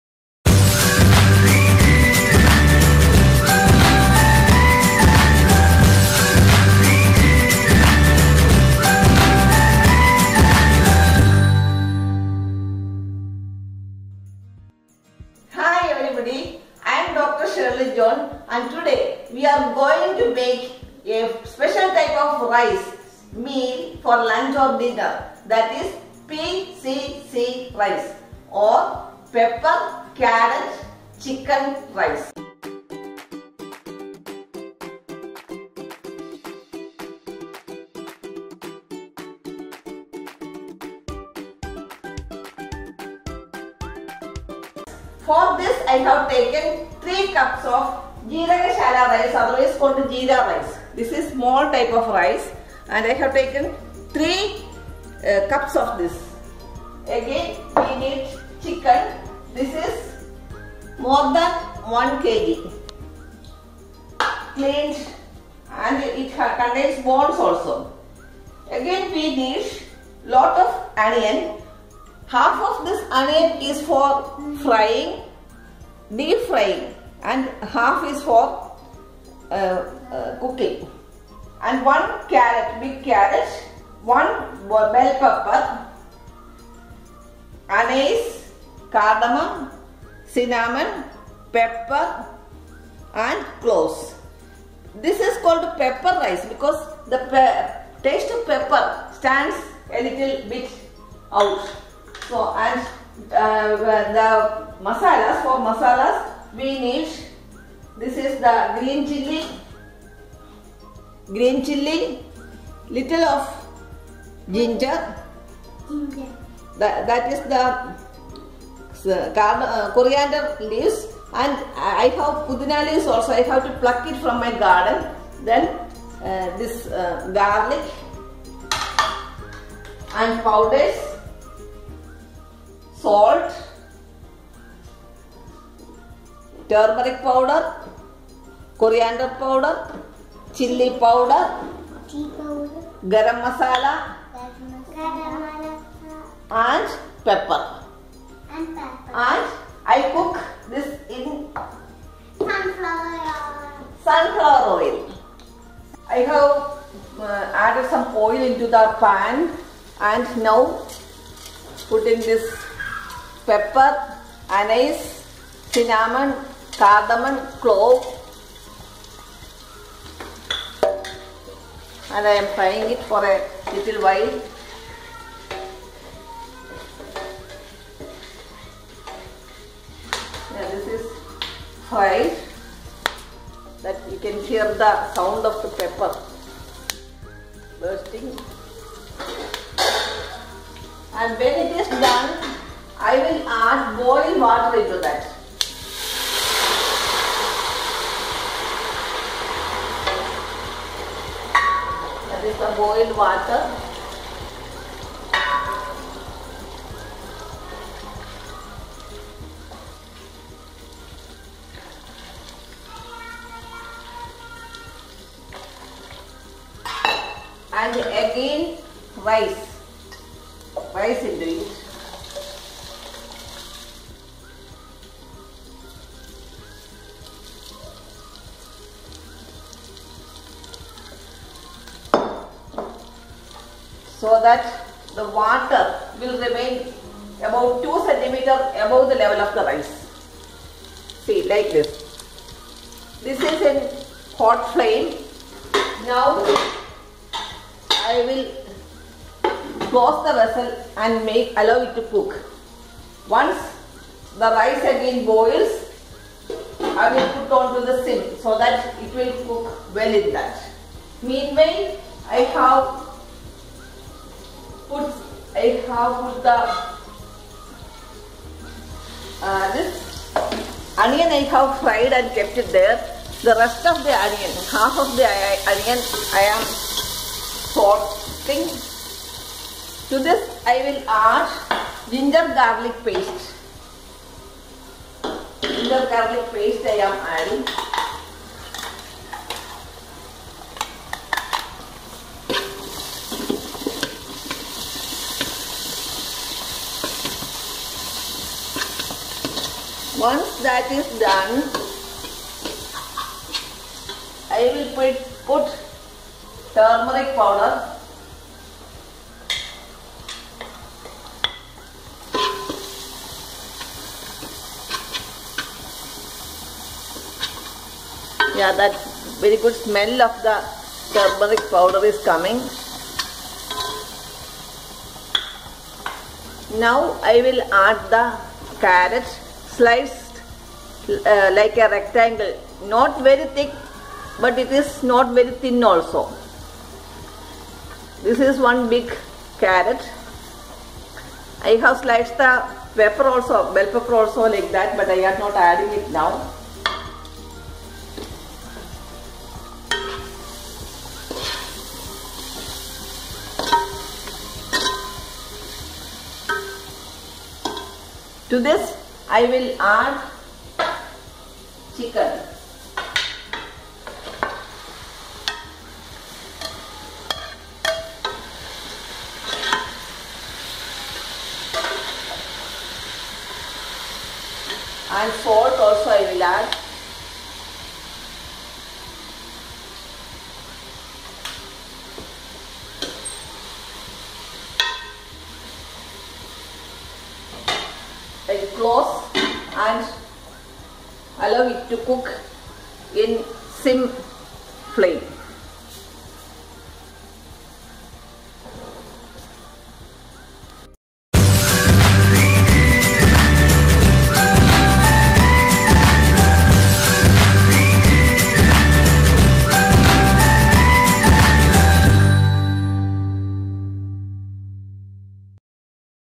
Dr Shirly John | How to make Pepper-Carrot-Chicken Rice | Pepper-Carrot-Chicken Rice at Home
Dr Shirly John from Cochin sharing her favorite recipe of the tasty and easy to make a Pepper-Carrot-Chicken Rice. Follow Dr Shirly John for easy steps and make a yummy Pepper-Carrot-Chicken Rice at your home.